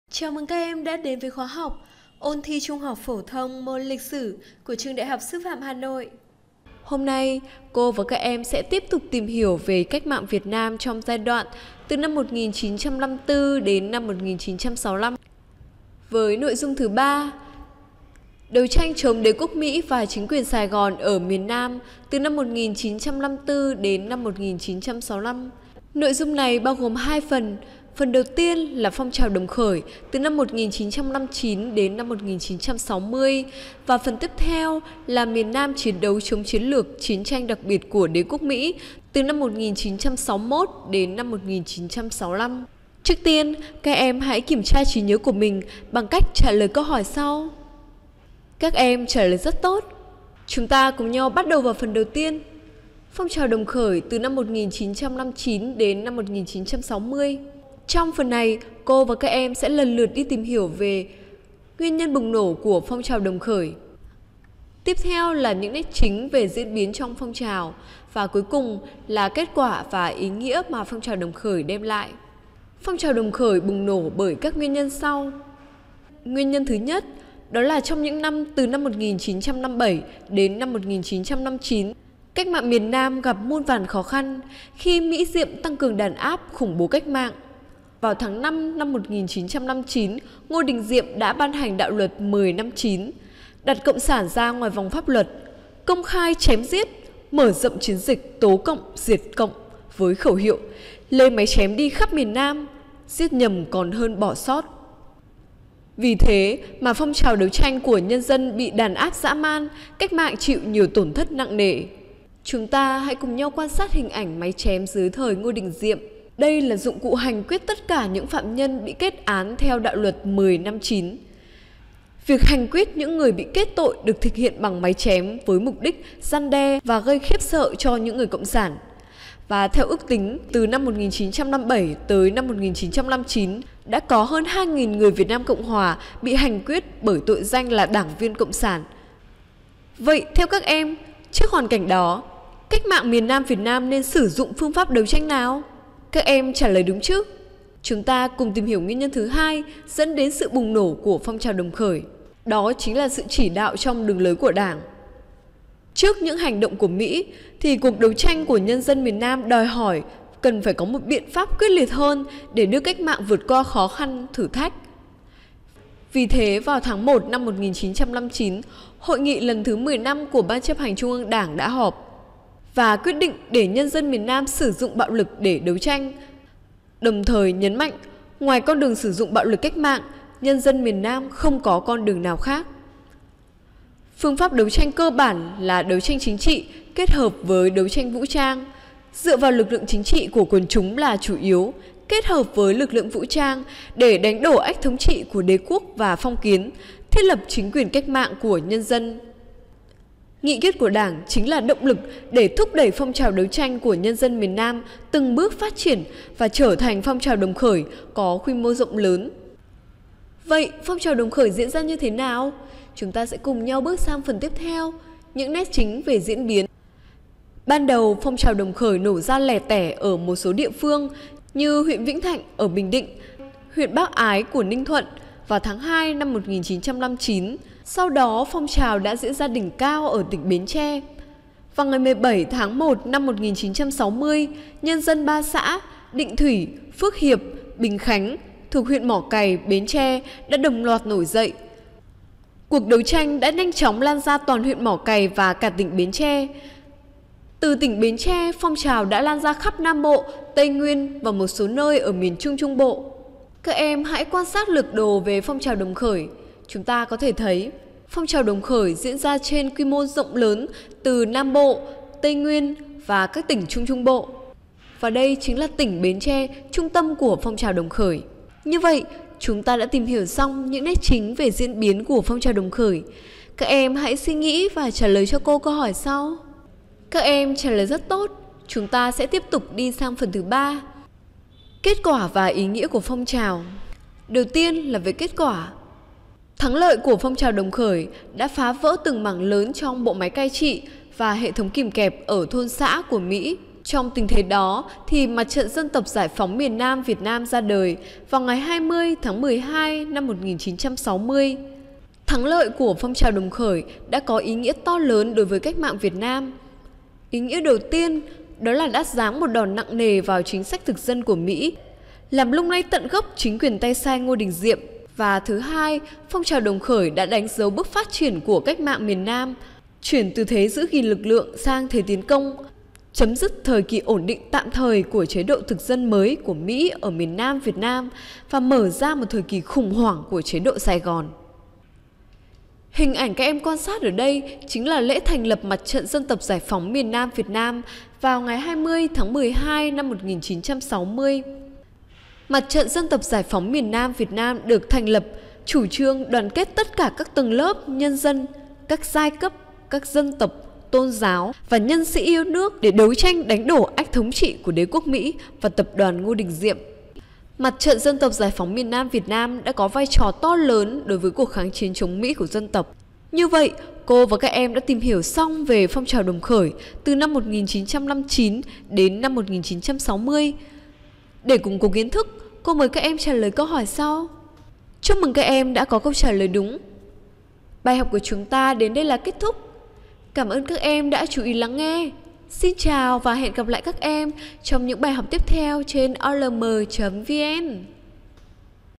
Phong trào Đồng khởi - Ôn thi THPT môn Lịch sử - OLM.VN -
Trong bài học này, các em sẽ tìm hiểu về:
- Nguyên nhân bùng nổ của phong trào Đồng khởi: đạo luật 10/59.
- Những nét chính về diễn biến: nổ ra tiêu biểu ở Bến Tre.
- Kết quả và ý nghĩa: cách mạng miền Nam chuyển từ thế giữ gìn lực lượng sang thế tiến công.